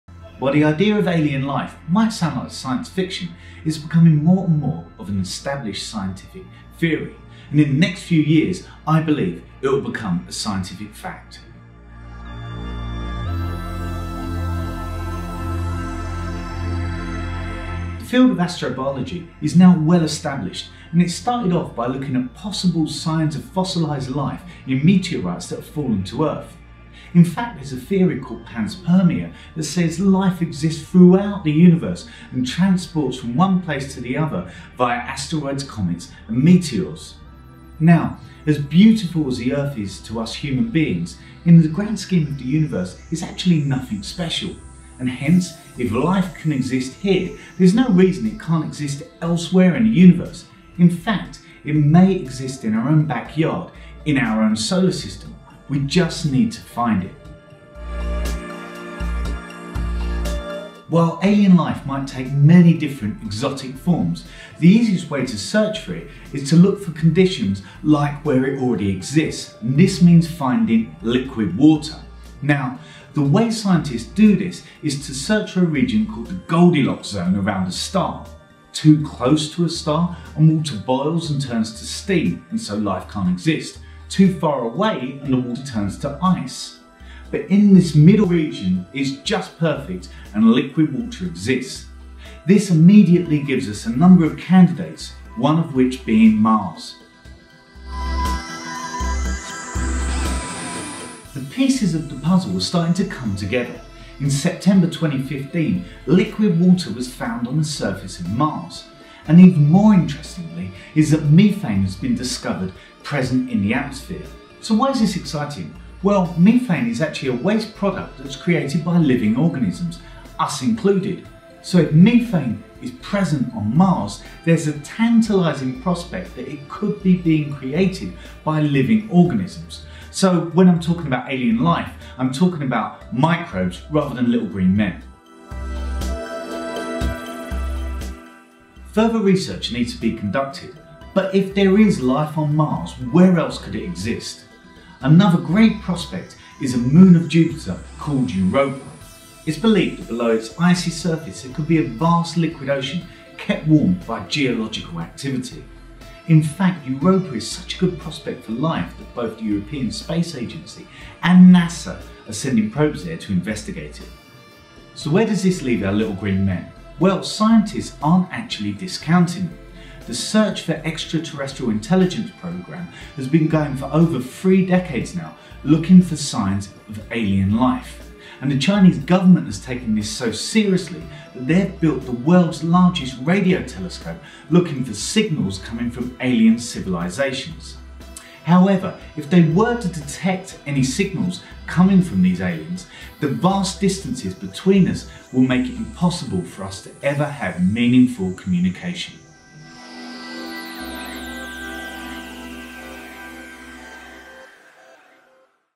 Does Alien life exist? | Space Explained
In this episode of ‘Space Explained’, Dr Simon Foster, a Solar Physicist at Imperial College London, explains whether alien life could theoretically exist on other planets, and how the existence of liquid water is key to this search.